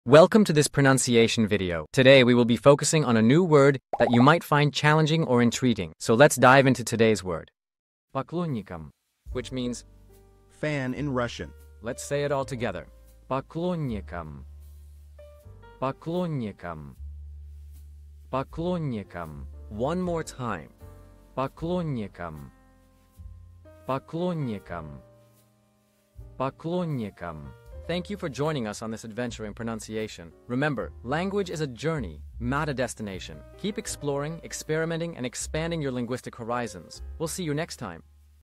How to pronounce Poklonnikom | Поклонником (Fan in Russian)
Master the Pronunciation of 'Poklonnikom | Поклонником' - which means : Fan in Russian 📝 with @PronunciationRussian 🎓🗣️ - [Your Guide to Russian]

Learn to pronounce 'Poklonnikom' perfectly in Russian with our easy and engaging tutorial by @PronunciationRussian! 📖📽️ In this comprehensive video, we provide you with native speaker examples 🗣️👂 and phonetic breakdowns 🎵 to ensure you get the pronunciation just right.